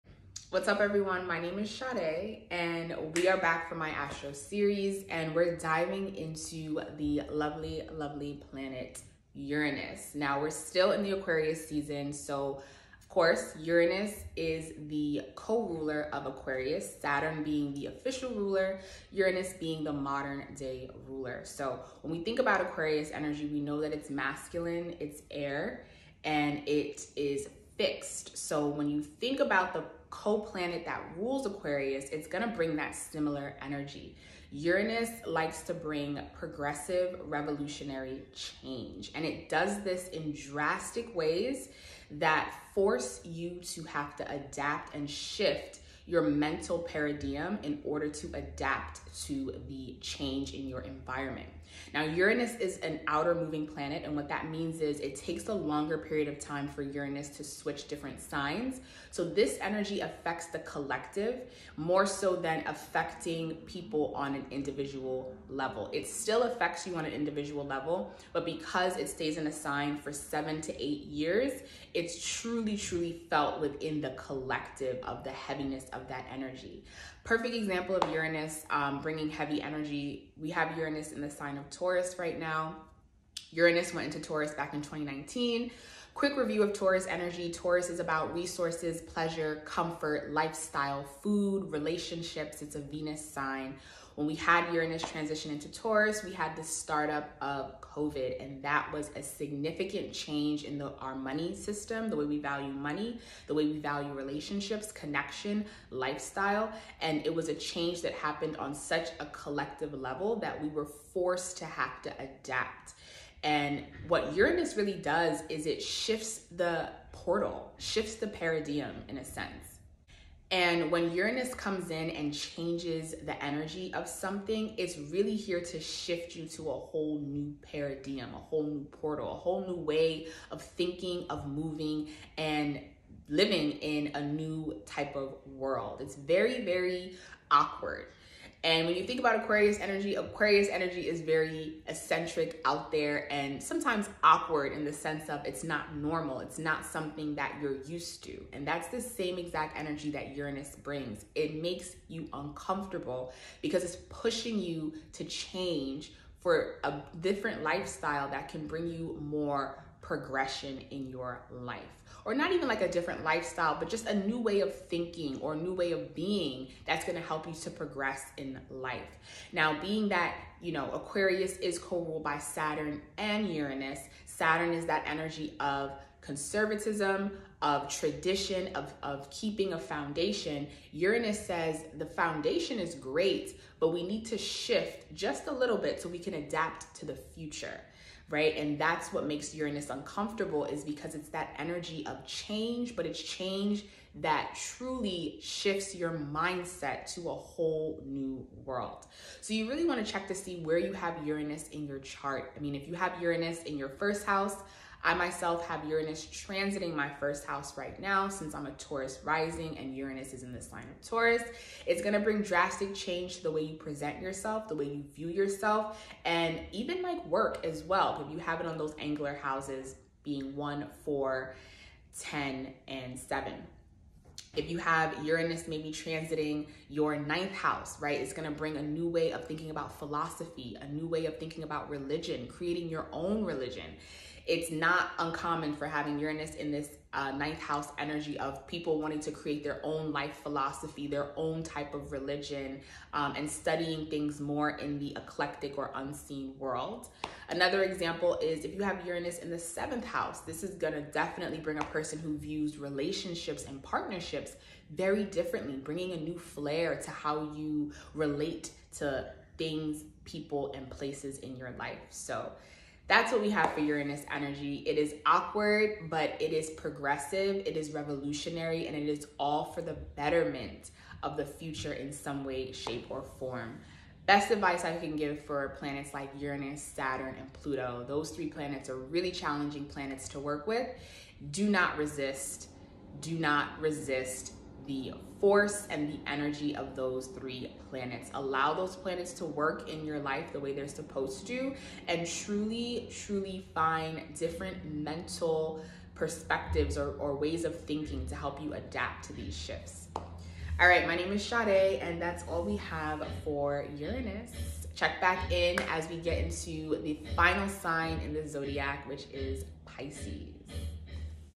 Uranus in Astrology
Uranus is the co-ruler of Aquarius. Uranus is all about progressive and revolutionary change that forces us to adapt to a new paradigm. Because it takes 7-8 years for Uranus to change signs, the heaviness of uranus energy is felt deeply on a collective level. Uranus is currently in the sign of Taurus until 2026 affecting how we relate to money, resources, relationships, food and overall lifestyle.